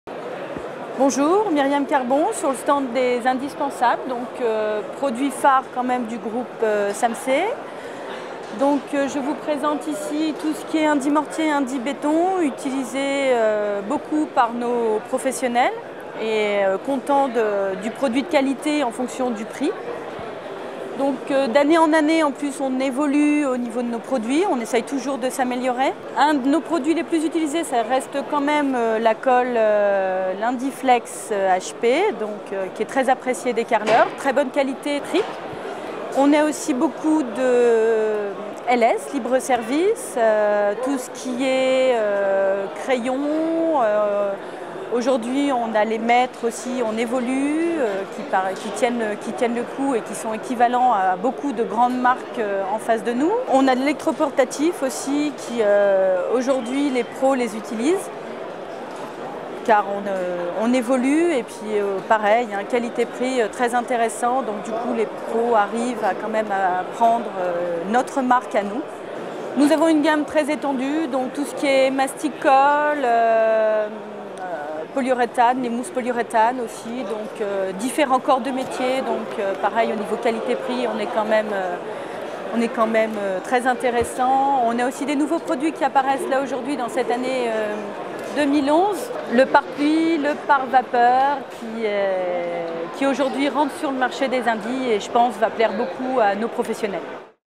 Les Indispensables - Produits de pros pour les pros (Samse)
SAMSE vous invite à découvrir LES INDISPENSABLES - la marque distributeur des produits de pros pour les pros.
Interview de Myriame Carbon.

Une gamme au meilleur rapport Qualité Prix : mortier colle, électroportatif, mastic colle .